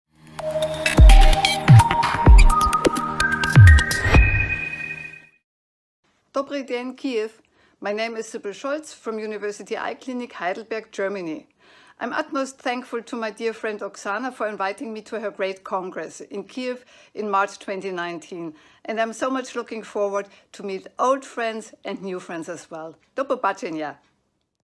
Sibylle Scholtz, MD, PhD
Доктор Сібіл Шольц представить на OphthalmicHUB 2019 інформацію про "простір Бергера" та його актуальність в сучасній хірургії катаракти.

Лише до 1 січня 2019 року діє більш ніж 30% знижка на вартість участі в науковій програмі.